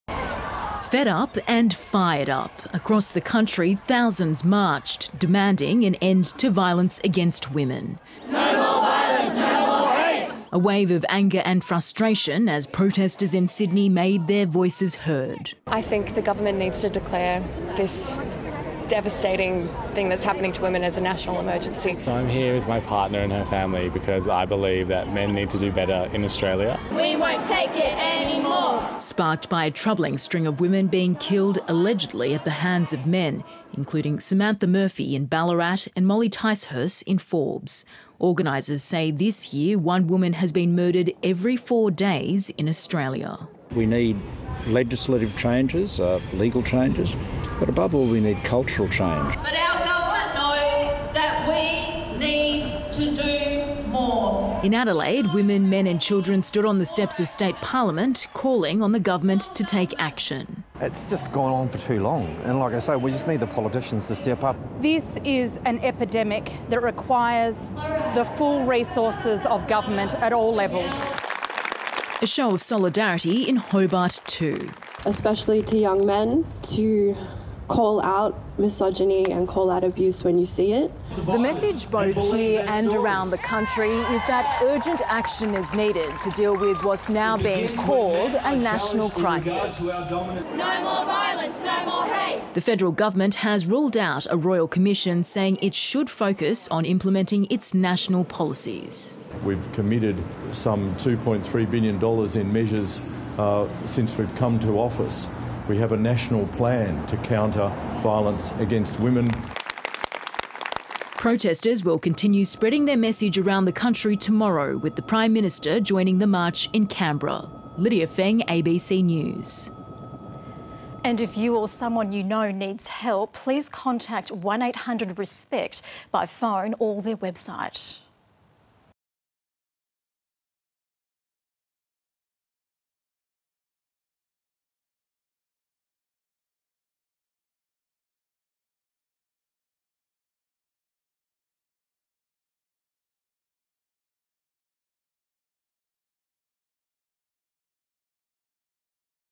Thousands gather across country calling for end to gender-based violence | ABC News
Rally organisers are warning the situation has reached crisis levels, with one woman being killed every four days. The government says it has a plan in place to counter the violence, but advocates want to see more urgency.